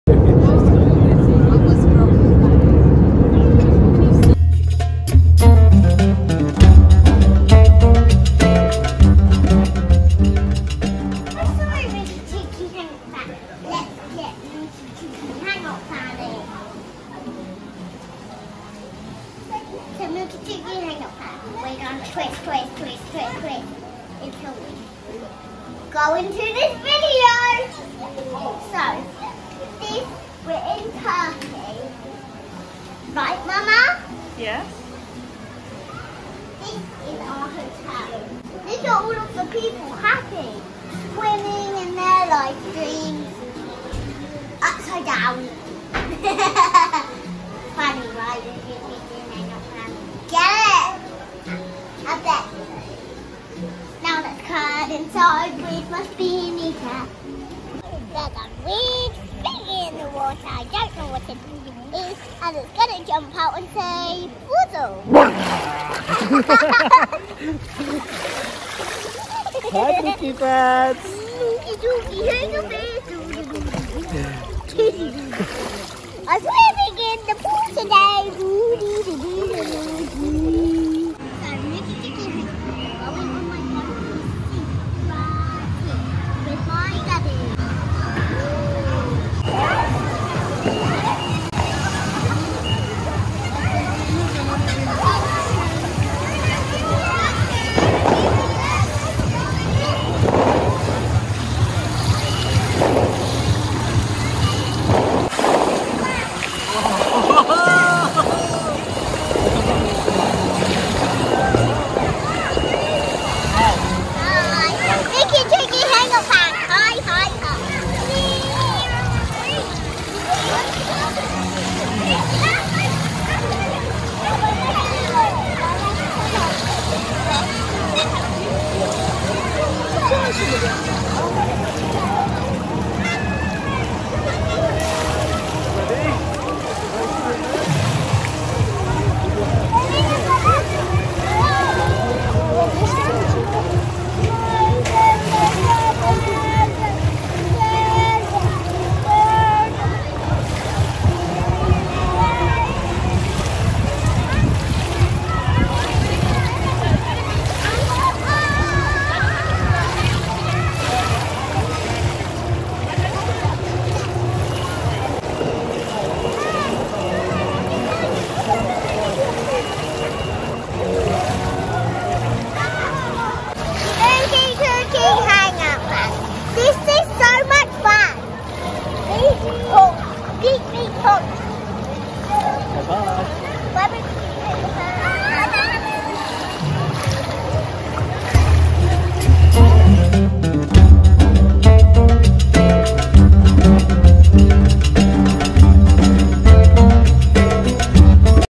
On Holiday | Swimming Fun | First Trip to Turkey!! | Family Holiday!
My first ever trip to Turkey, lots of fun in the pool!